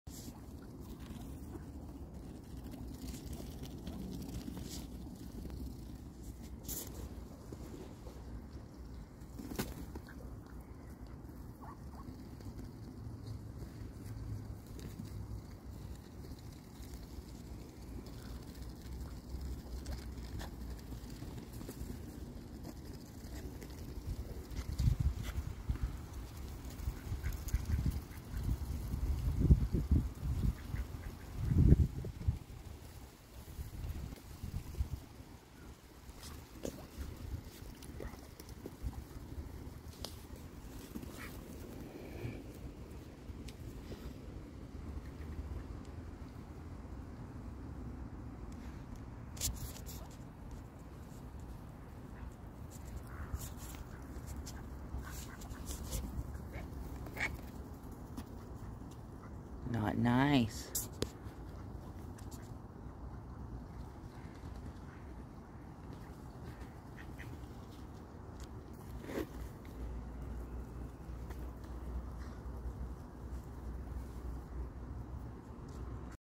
Chilling with my Darlings
I absolutely love all The ducks But some of them love me back.